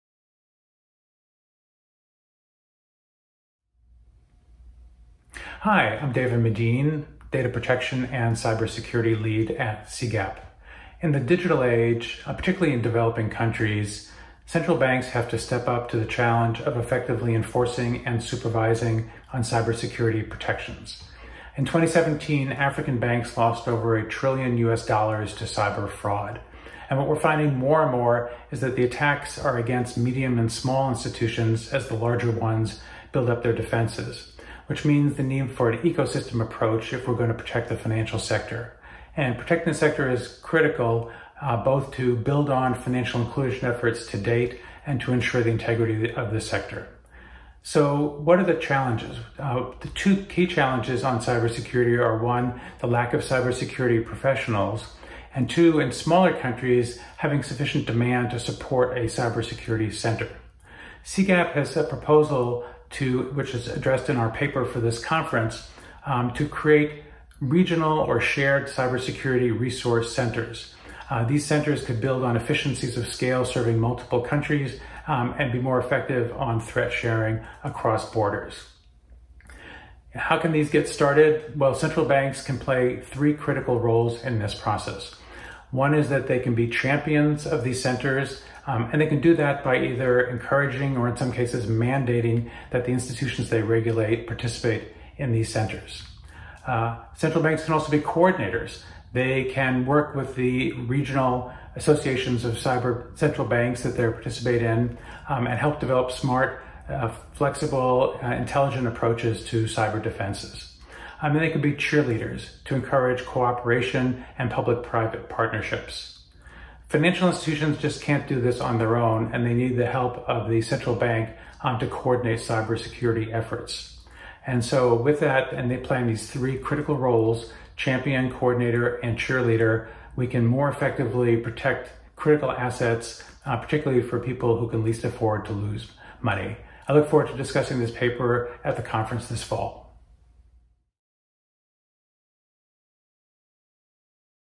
Developing Country Central Banks of the Future as Cyber Security Champions/Coordinators/Cheerleaders
David Medine, CGAP's Data Protection and Cybersecurity Lead, proposes the creation of regional cybersecurity resource centers to help small countries collaborate on threat identification and mitigation. He argues for a pivotal role for central banks in the development and growth of these centers.